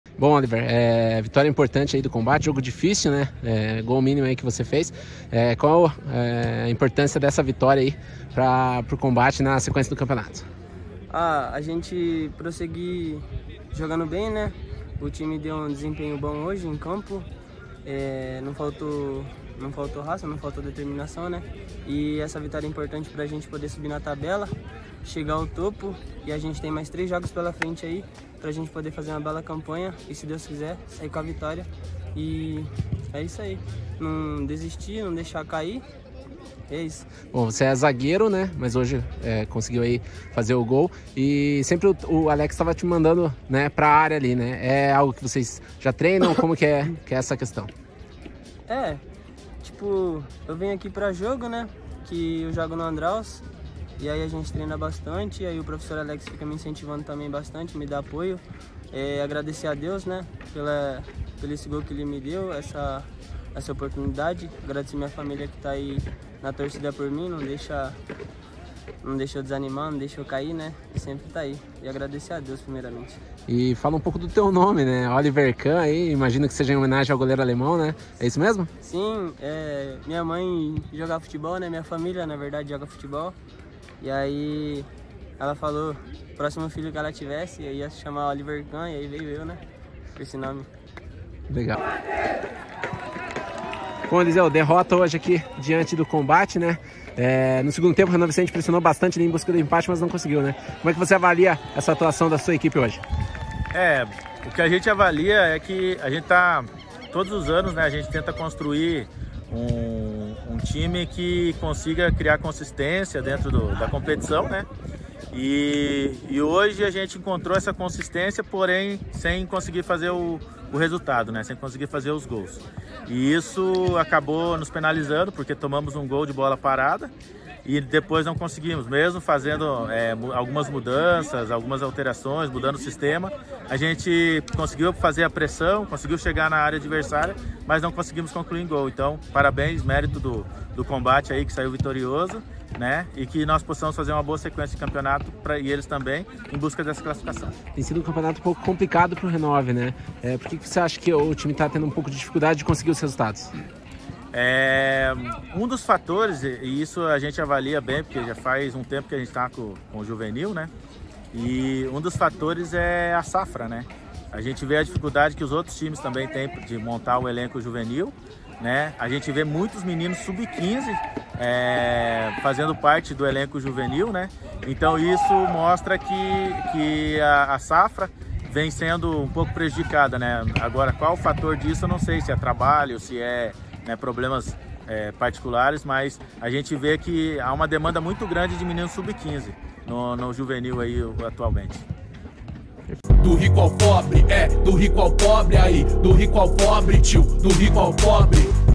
[CP] - COMBATE 1 X 0 RENOVICENTE [6ª RODADA - GRUPO B | SUBURBANA SÉRIE B | JUV
Confira as entrevistas realizadas no sábado (22) no Recanto Tricolor, Barreirinha, após a partida entre Combate Barreirinha e Renovicente, válida pela sexta rodada do grupo B da Série B da Suburbana 2023, categoria juvenil. O repórter Yuri Casari entrevistou o zagueiro Oliverkahn (Combate) e técnico Elizeu Duarte (Renovicente).